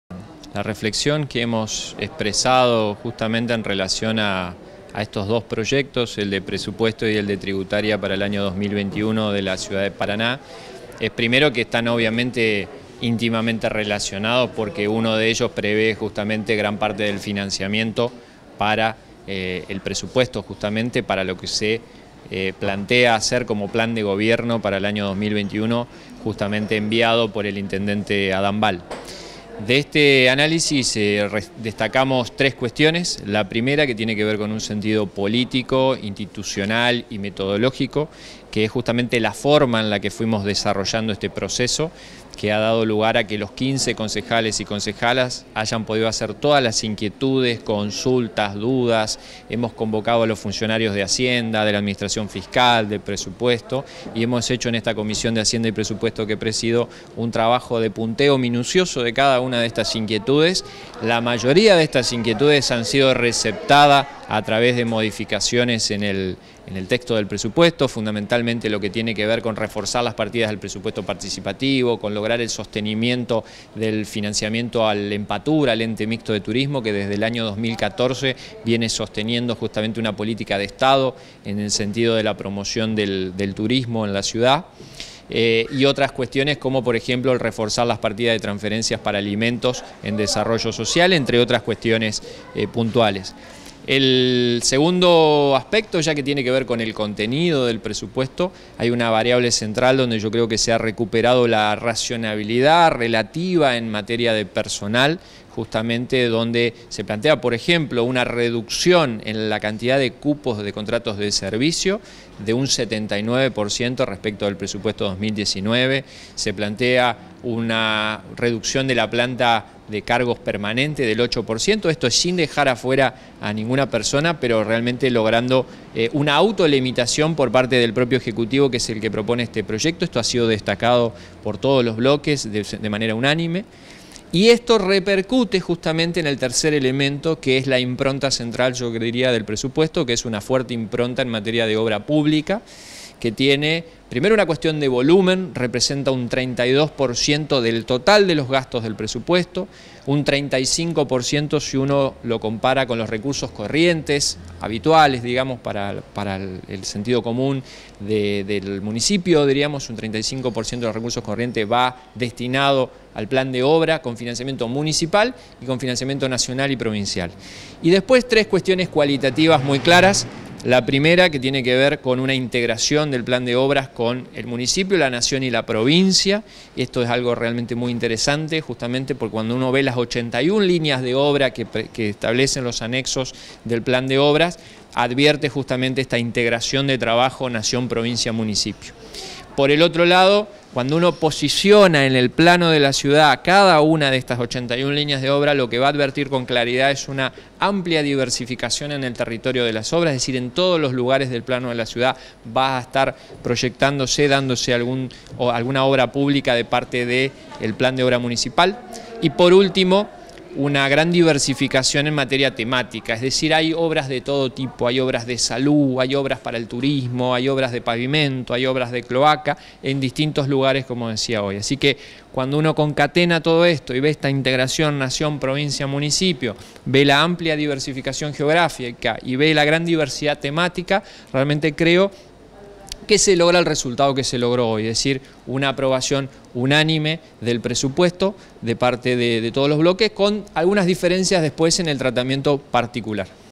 Se aprobó el Presupuesto 2021
El concejal Sergio Granetto, presidente de la Comisión de Hacienda del HCD, destacó en primer lugar los tiempos y metodología participativa en el tratamiento de la norma. Luego la reducción en el ítem correspondiente al personal restableciendo una racionalidad en la materia; sin que esto implique afectar la estabilidad de los trabajadores, se reduce el cupo de contratos de servicios en un 79%
Por último resaltó la partida destinada a Obra pública que implica un 35% de los gastos corrientes del municipio y se traduce en obras en toda la ciudad.